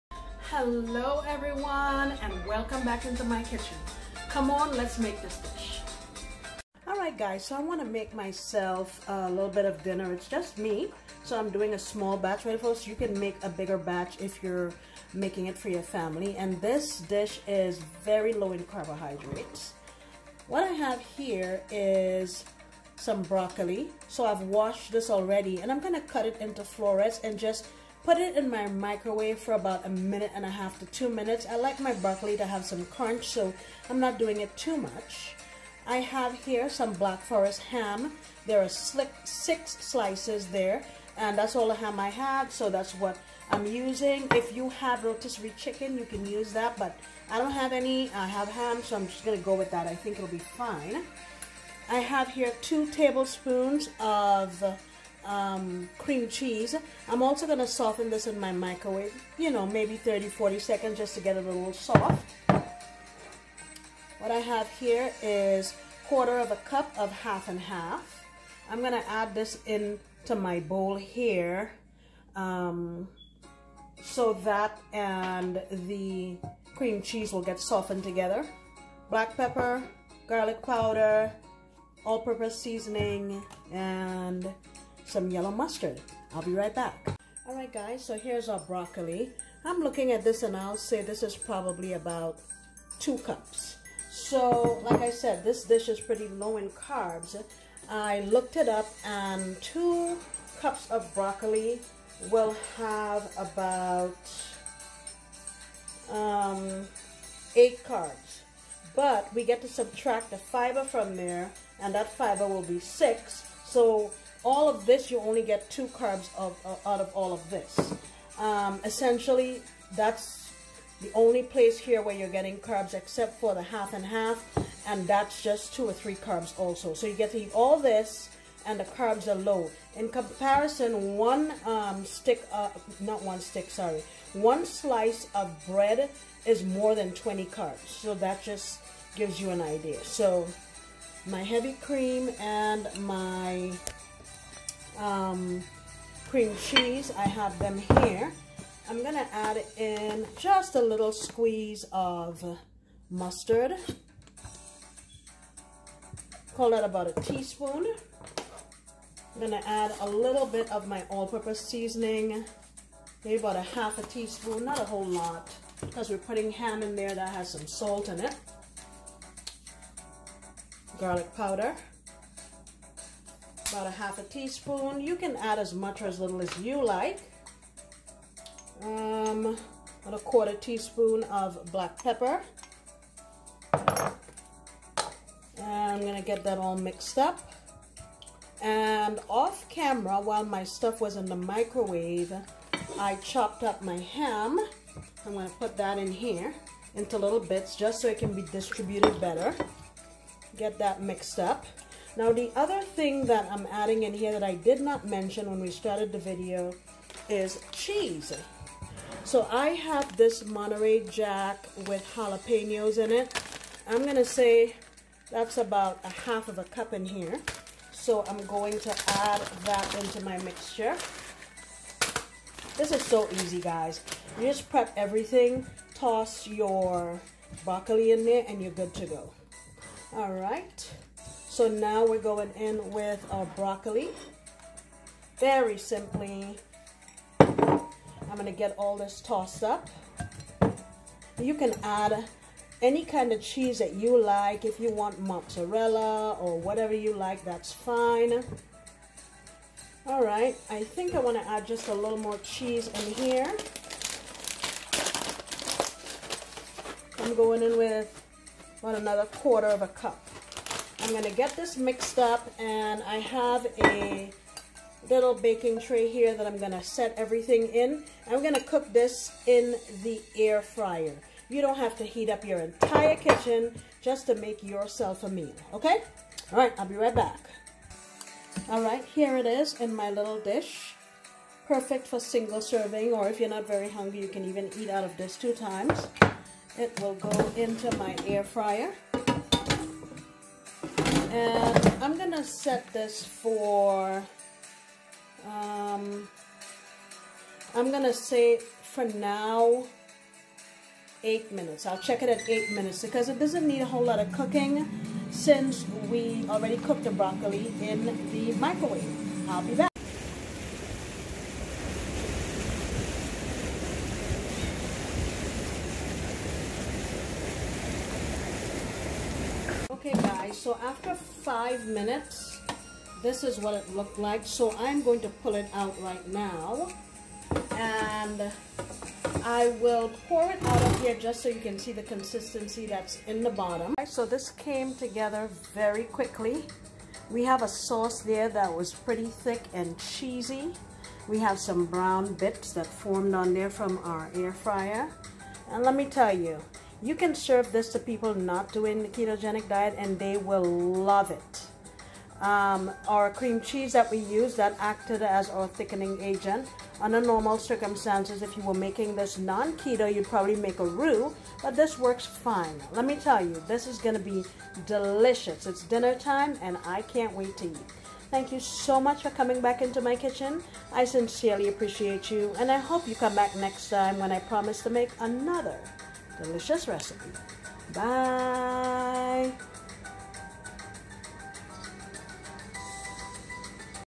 Delicious Dinner in Under 10 Mins ~ Episode 350
Hello everyone and welcome back into my kitchen. Today we will be making a creamy, delicious dinner in under 10 minutes. This dish is low in carbs, but will be sure to delight any palate. Join me